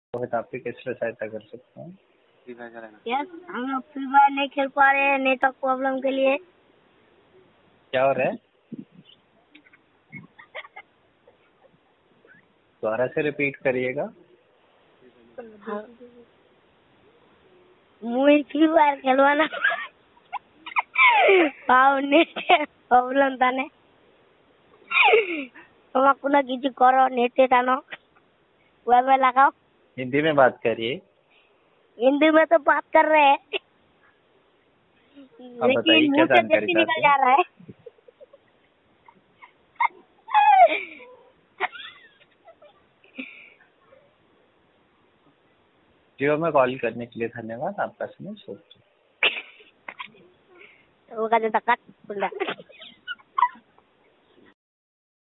Jio customer care prank call/Free Fire nehi chal raha hain/ Jio customer care prank in hindi
Jio customer care prank call